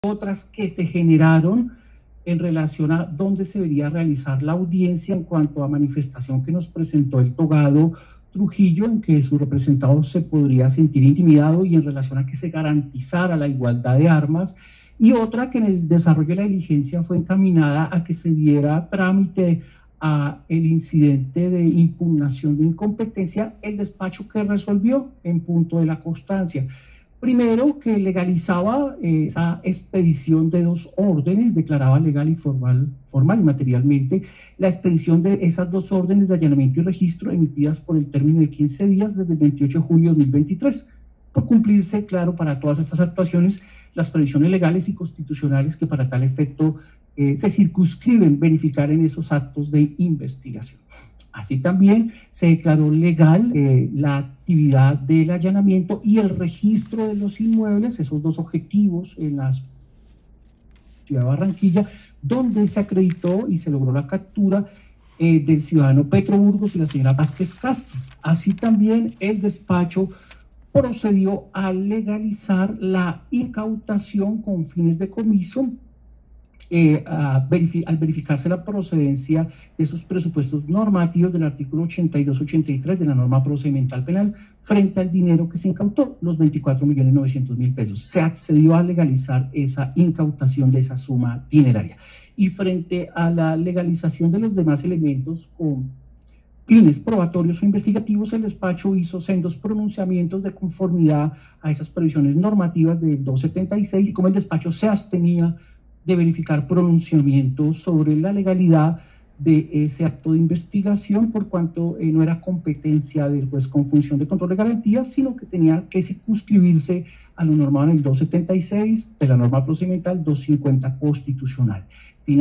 La Fiscalía de Colombia imputa cargos a Nicolás Petro y Day Vásquez
Ante un juez de garantías, la Fiscalía presenta las pruebas en contra de Nicolás Petro y Day Vásquez. El mismo juez legalizó la captura y los allanamientos para detener al hijo del presidente Gustavo Petro.
Sin Filtros 1 de agosto de 2023

Gettr. @Maibort Petit

Ayúdanos comprando nuestros libros en Amazon